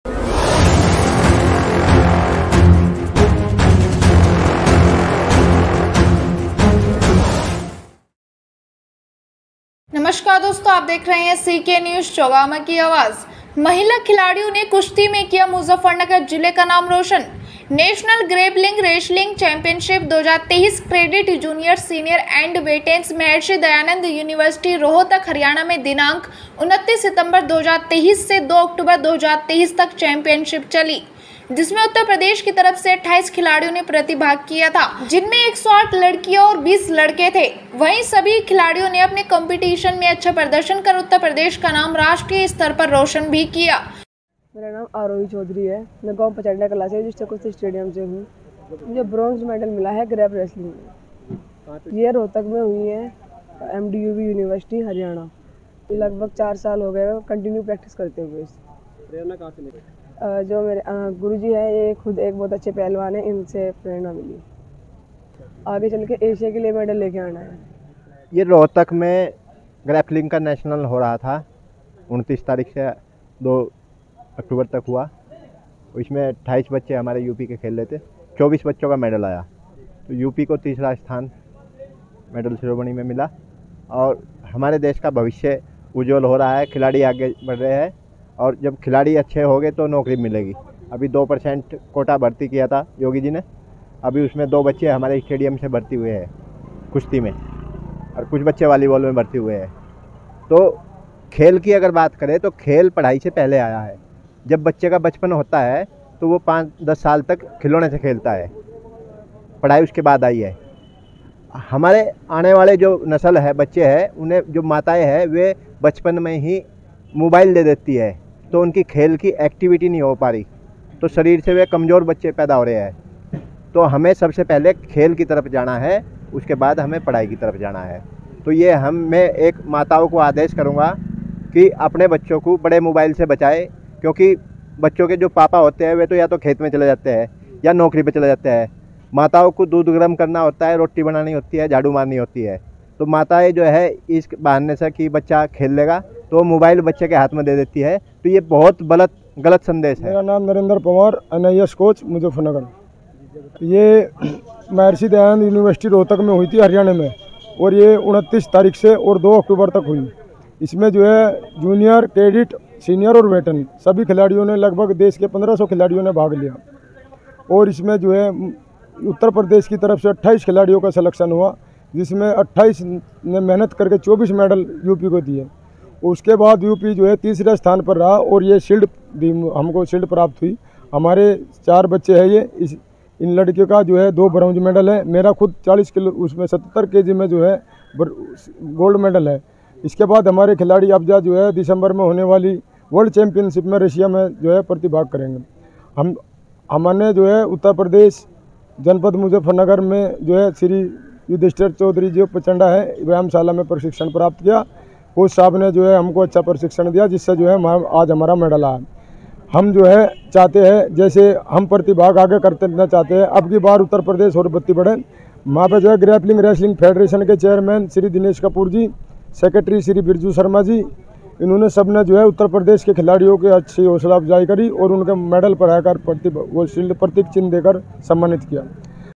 महिला खिलाडियों ने कुश्ती में किया मुजफ्फरनगर जिले का नाम रोशन#Wrestling#Muzaffarnagar#femaleplayers
महिला खिलाडियों ने कुश्ती में किया मुजफ्फरनगर जिले का नाम रोशन